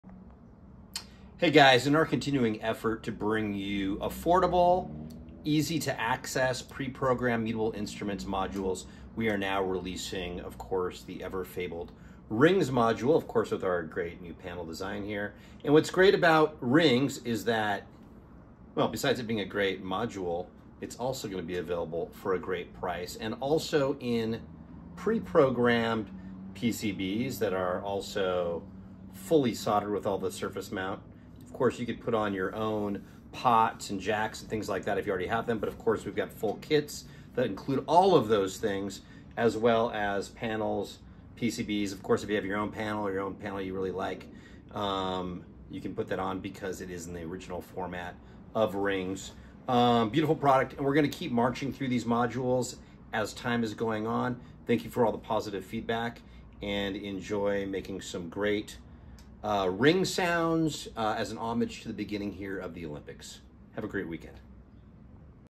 RINGS - Mutable Instruments Clone kits, PCBs and completes - out now!
Rings is a resonator, the essential ingredient at the heart of several physical modeling synthesis techniques. The resonator transforms an external, unpitched excitation audio signal into a full-bodied pitched sound. Rings is the bar, the tube or the bunch of strings you cause to vibrate with a bursty external signal.

Any signal can be fed into the resonator, including envelope clicks, trigger signals, noise bursts, even tonal sounds. It also contains a special "polyphonic mode", where four notes can ring together at the same time. Although it is not truly polyphonic, it adds density and chordal characters to the sounds output. For those wanting the functions of Elements without the losing HP real-estate, Rings is exactly the solution.

Three families of vibrating structures are simulated by the module, with CV control over their parameters:
Strings, membranes and tubes as modeled by Elements’ resonator section (modal synthesis).
Strings coupled together and vibrating in sympathy, with controllable intervals between them.
Strings with a variable amount of inharmonicity.
Rings can be configured so that each new note is played on its own virtual string, while the previously played note(s) still decay. This unique take on polyphony allows the module to play strummed chords.